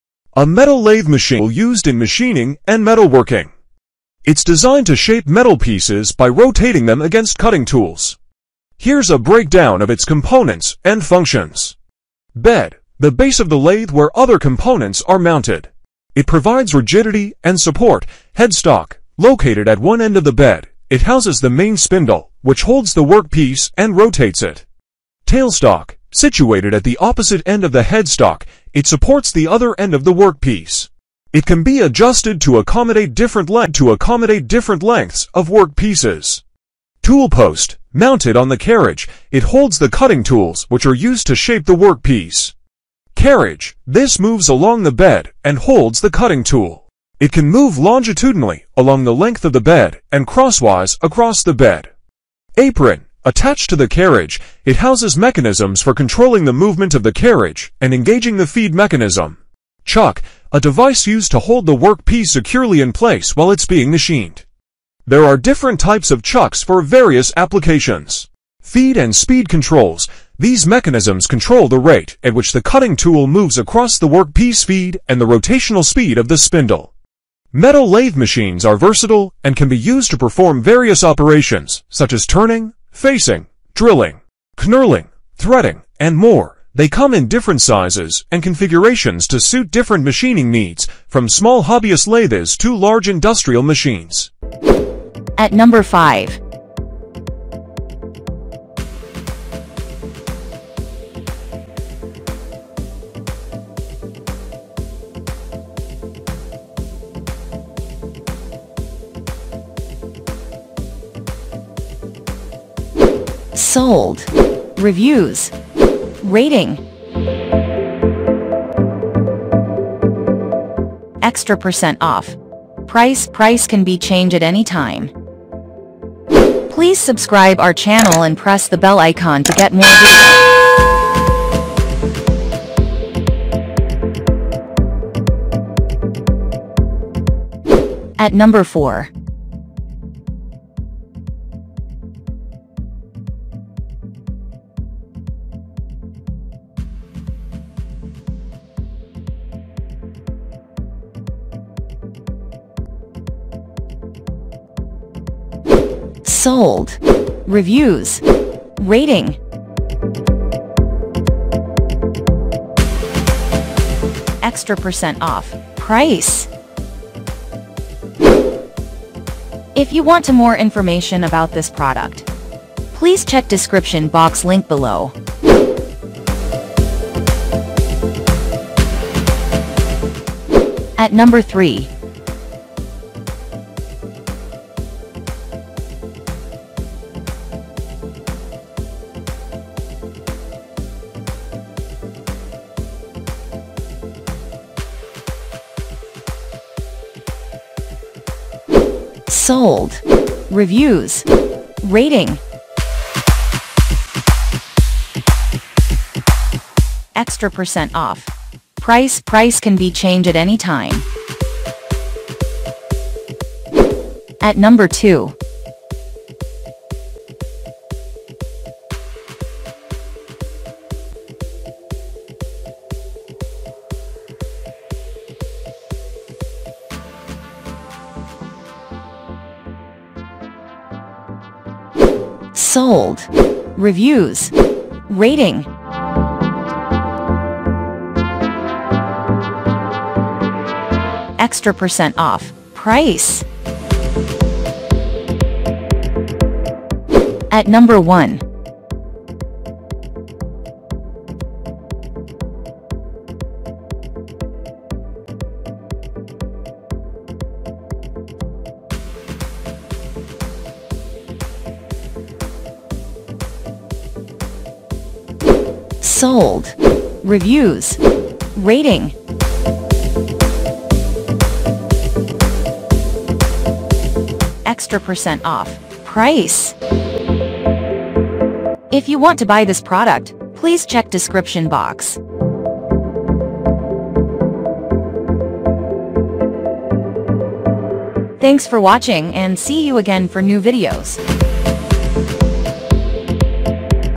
TOP 5 Best Metal Lathe Machine Review In 2024
Check The Link for Buy & Details:
1. VEVOR Metal Lathe Variable Speed 1100W 750W 650W 550W Lathe 220V 110V For Precision Counter Face Turning Driling Making Threads

2. 210V Mini Metal Lathe Machine Multi-function Woodworking Metal Small Machine Tool 900W/1100W CNC Lathe

3. Mini Metal Lathe,Metal Lathe Machine 1100W 220MMx750MM Variable-Speed Metal Lathe RPM Metal Milling Machine Digital Milling

4. Mini Metal Lathe Machine 15-3/4" 1100W 300-800MM 50-2500 RPM Variable Speed 125mm Chuck Scale Optional For Metalworking Drilling

5. HMT-600A Lathe Metal Processing Mechanical Multi-function Lathe Machine Tool 220V Woodworking Ordinary Micro Machine Tool 1PC